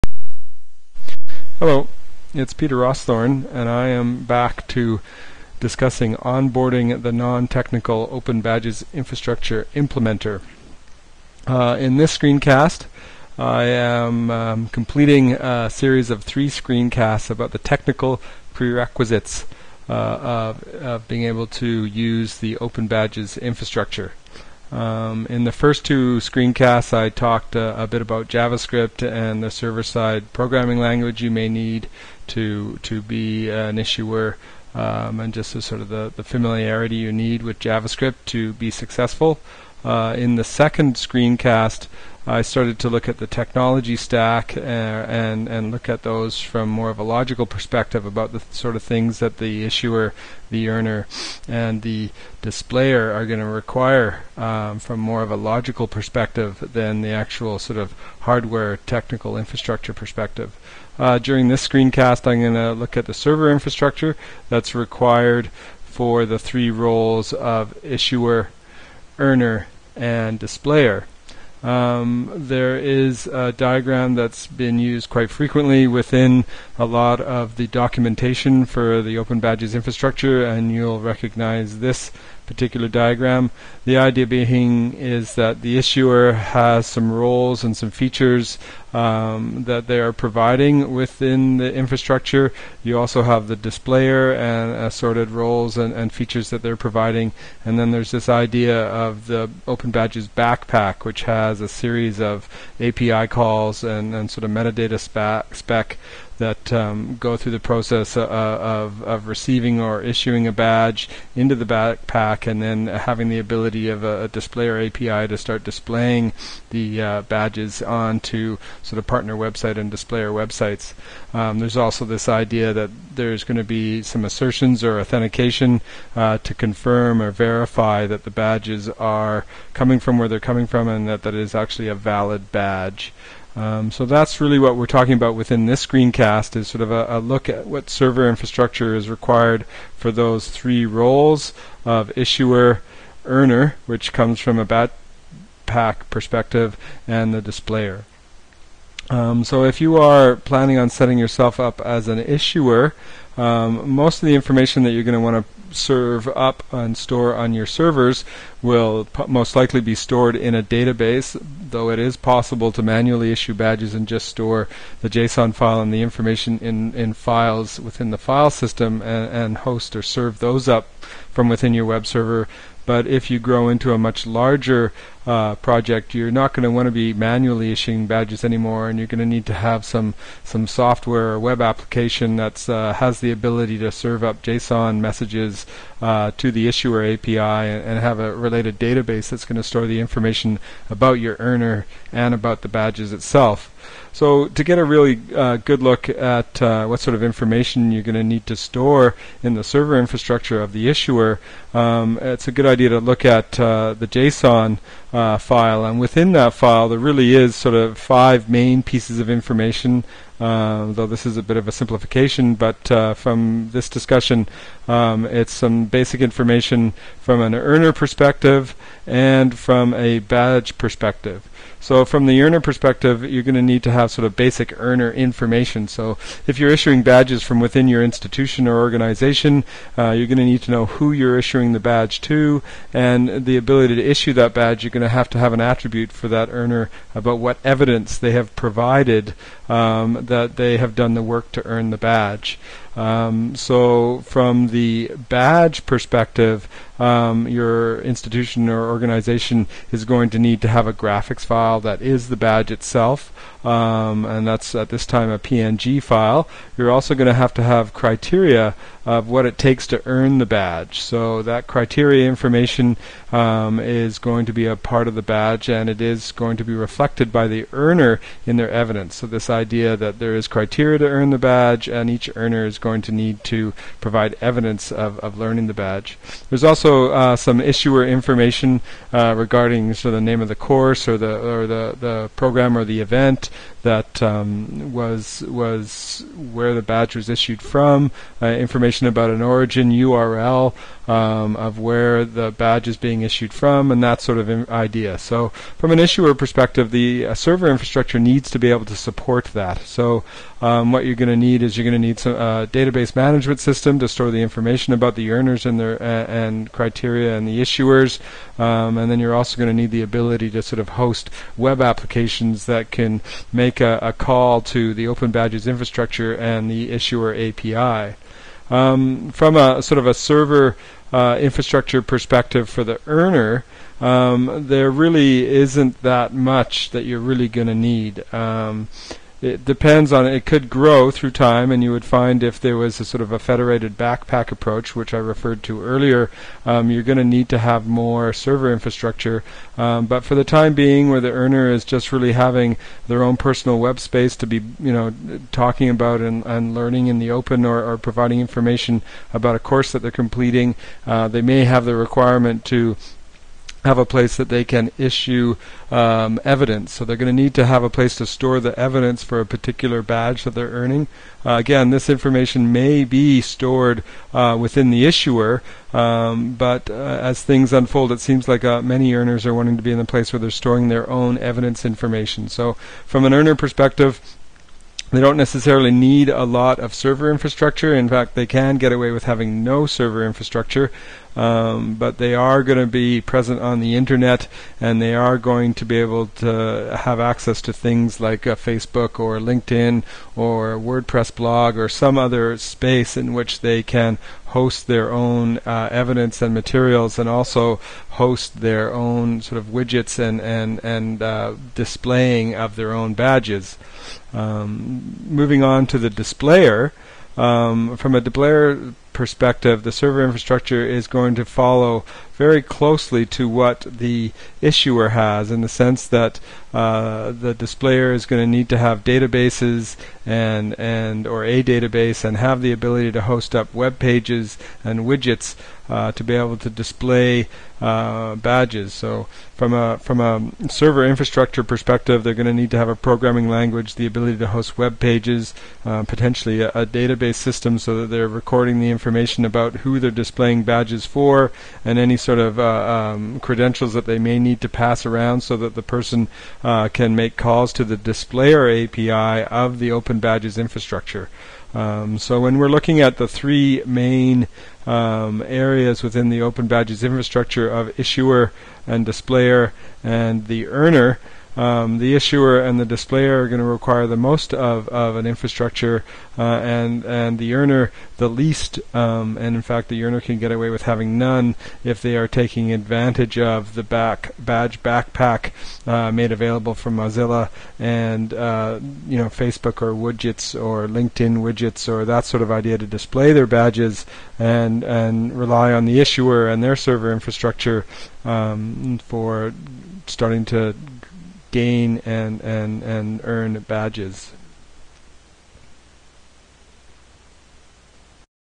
Open Badges Technical Prerequisites - Server Infrastructure
The second of three screencasts taking a look at the technical prerequisites for open badges. This screencast focuses on the server infrastructure prerequisites.